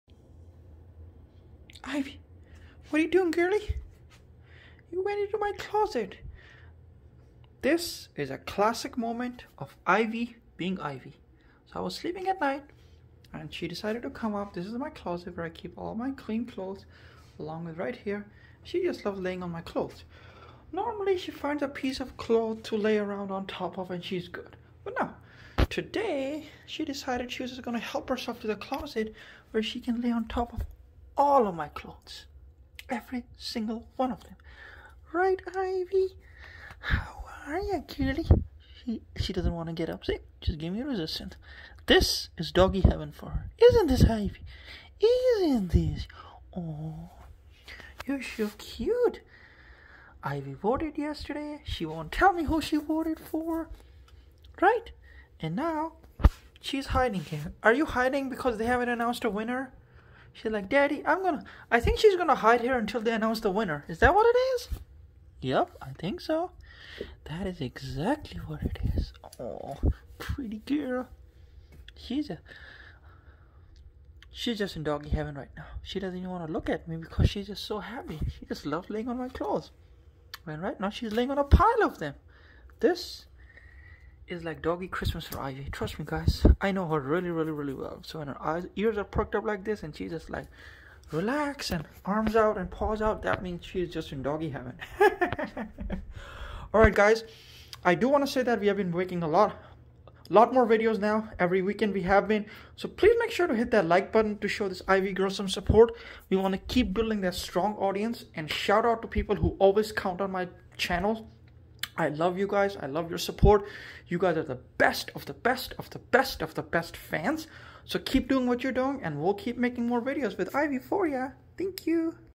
Blind Husky Dog Ivy creates her own bed
ivy being Ivyyyy